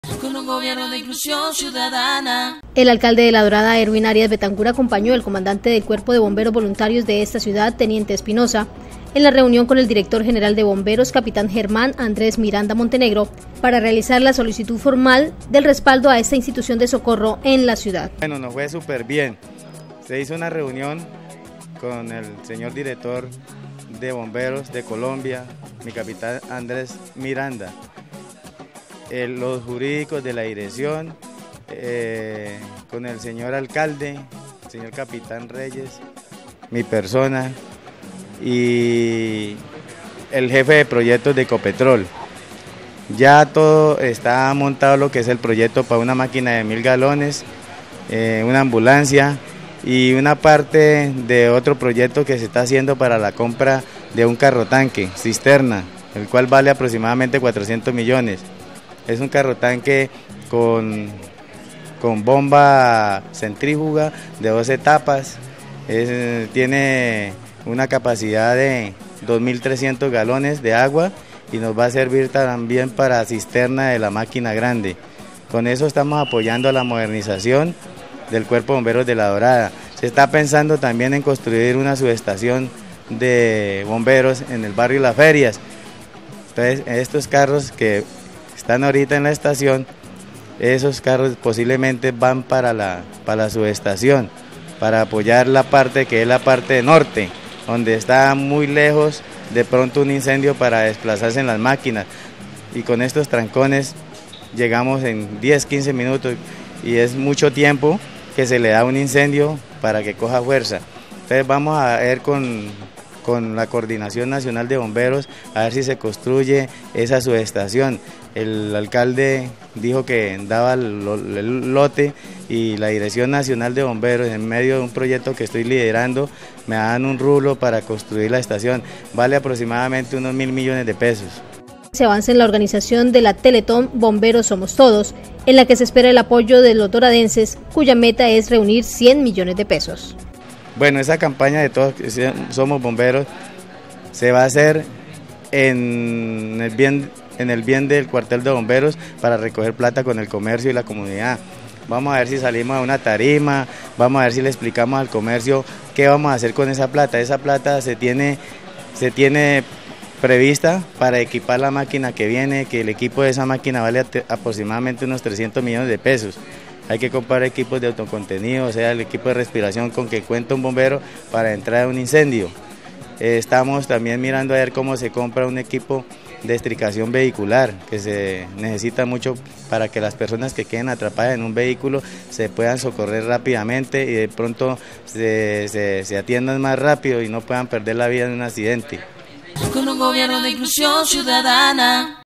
Alcalde La Dorada apoya gestión para Cuerpo de Bomberos
Erwin Arias Betancur, alcalde La Dorada
Elkin Alfonso Bustos, Dir. Prensa y Comunicaciones